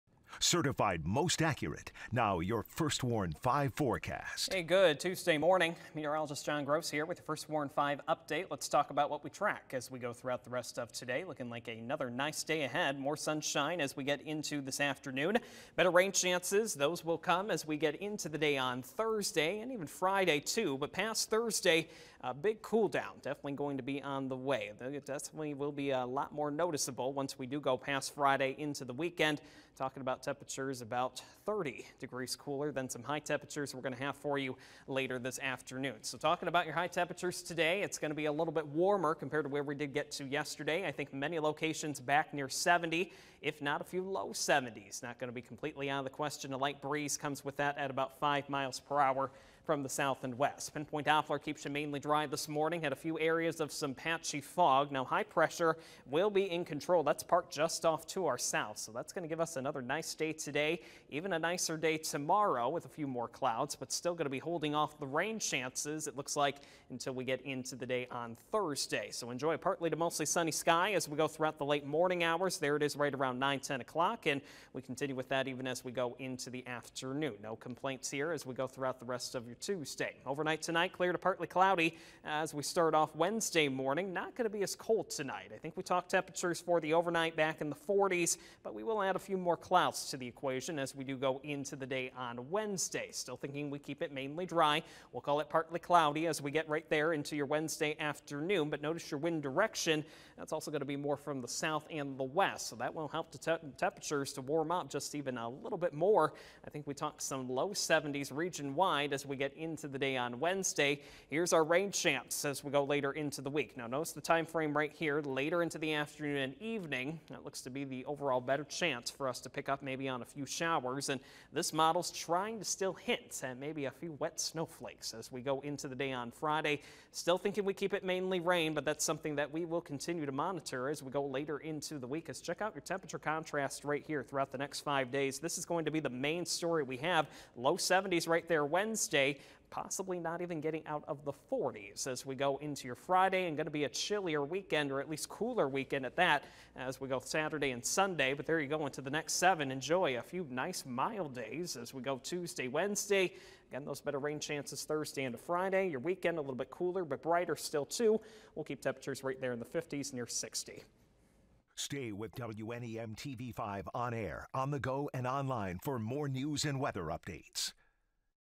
First Warn 5: Tuesday Morning Forecast, Oct. 4th
Meteorologist John Gross has your Tuesday morning forecast.